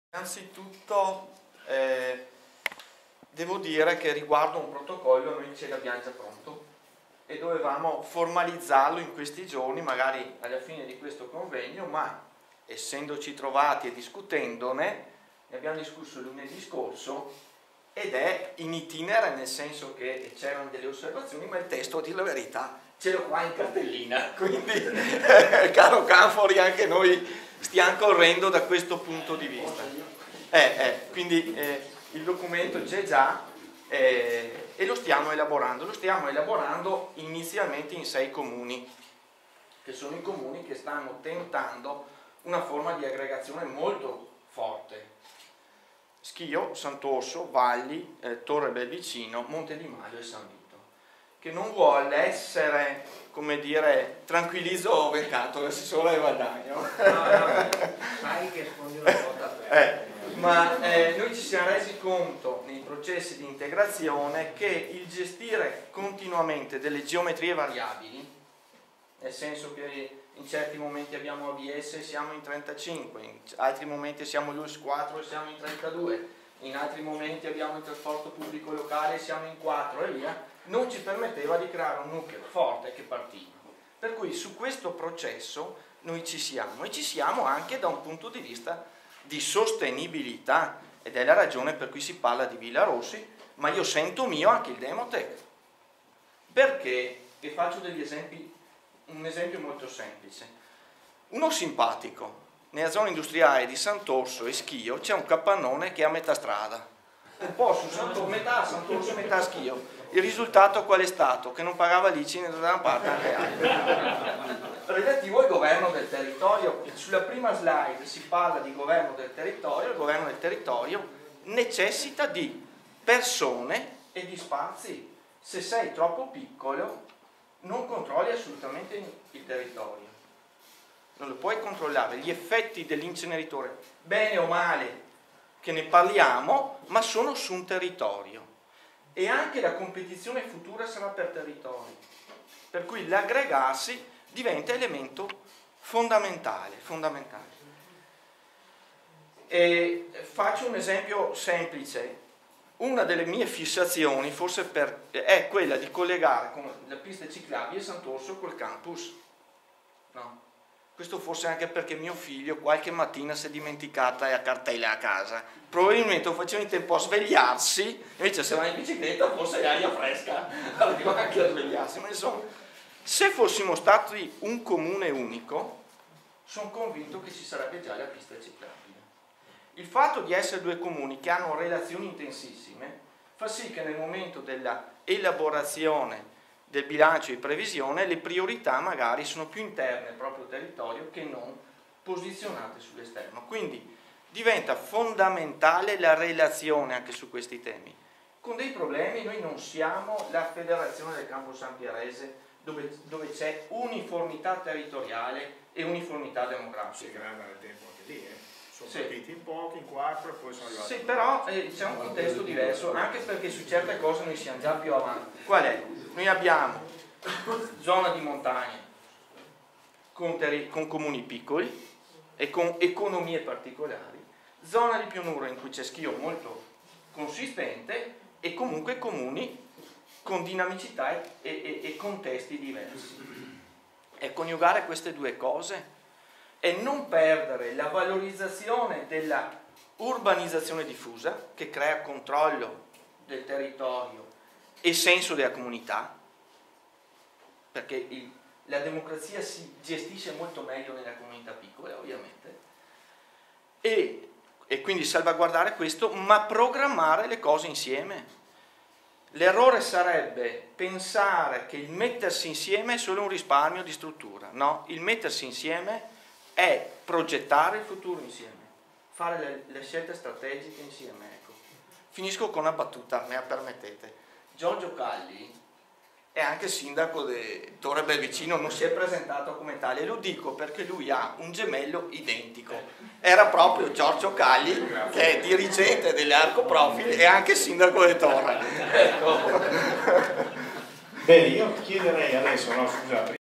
Costruire le sinergie locali a partire dai bisogni del territorio - Pietro Menegozzo
PIETRO MENEGOZZO Sindacco di Santorso

Santorso, piccolo comune ai piedi delle Prealpi Vicentine, è al centro di un territorio intercomunale caratterizzato dalla produzione manifatturiera e artigianale. Villa Rossi, già residenza di Alessandro Rossi, e il suo parco concepito come un podere modello, possono rappresentare lo spazio nel quale immaginare forme di promozione e attivazione sul territorio della sostenibilità ambientale.